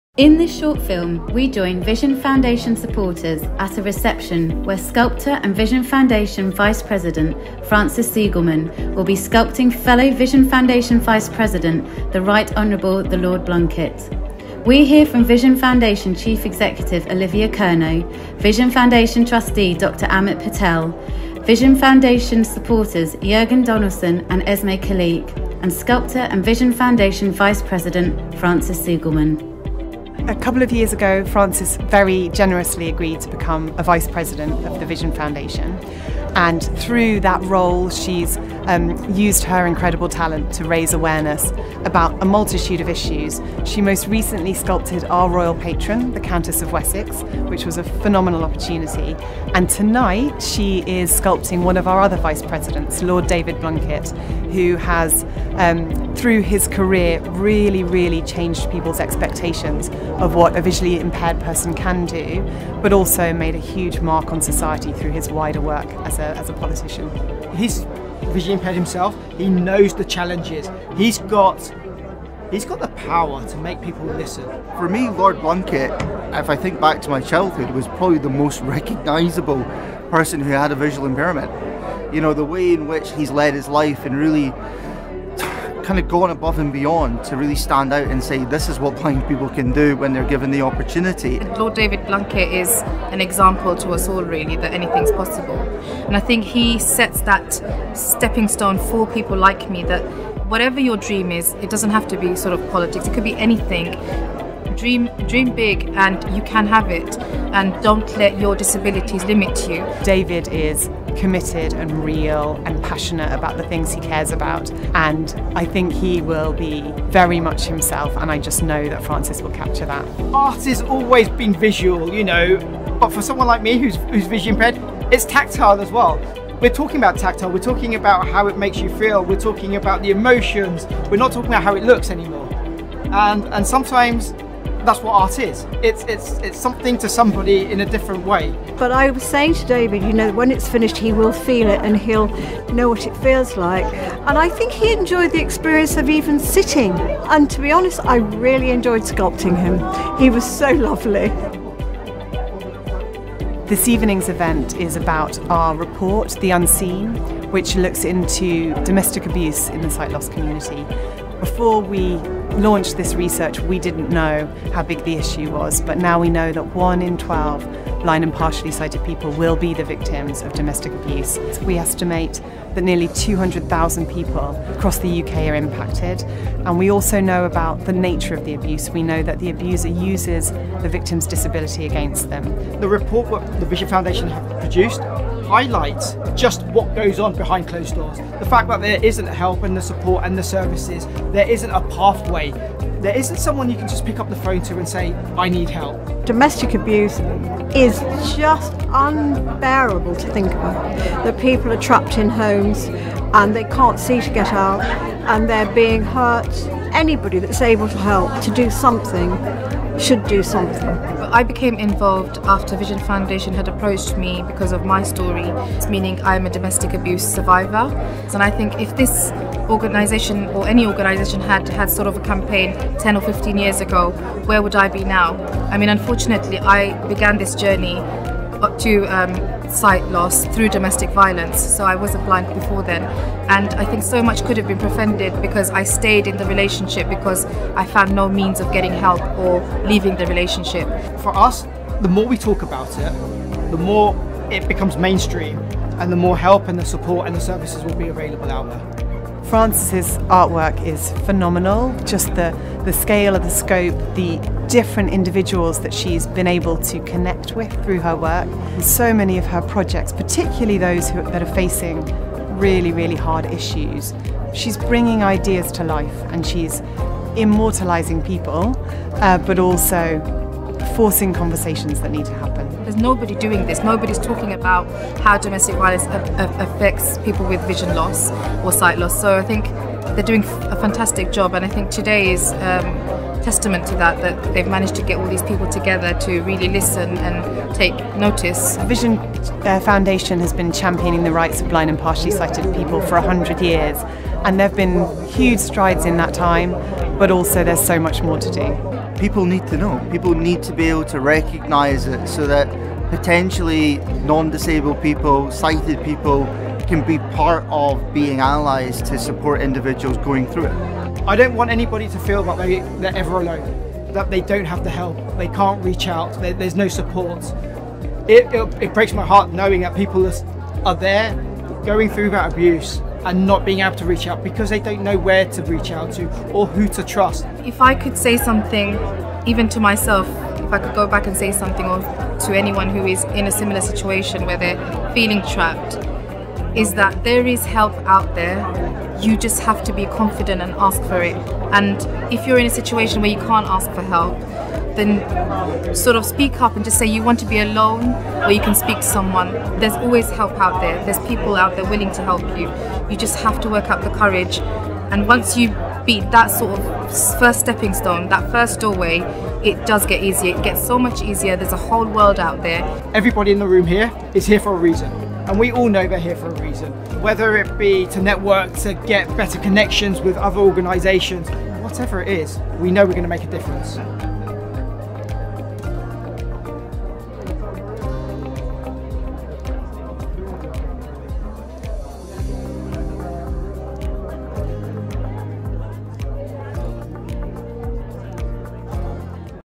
Sculptor Frances Segelman sculpts Lord David Blunkett for Vision Foundation
In November 2022, Vision Foundation welcomed parliamentarians, charity representatives and activists to witness renowned sculptor, Frances Segelman create Vision Foundation’s Vice President, Lord David Blunkett’s, likeness in clay at an intimate event in London.

At the event, we shared the findings of our recent report, The Unseen: blind and partially sighted people's experiences of domestic abuse.

In this short film, we join Vision Foundation supporters at a reception where sculptor and Vision Foundation Vice-President, Frances Segelman will be sculpting fellow Vision Foundation Vice-President, The Rt Hon. the Lord Blunkett. We hear from Vision Foundation Chief Executive Olivia Curno, Vision Foundation Trustee Dr Amit Patel, Vision Foundation Supporters Jurgen Donaldson and Esme Khalique, and Sculptor and Vision Foundation Vice-President, Frances Segelman.

Video description: Various shots of guests at the event. They are in conversation, listening to the event speakers or watching Frances Segelman sculpt Lord Blunkett.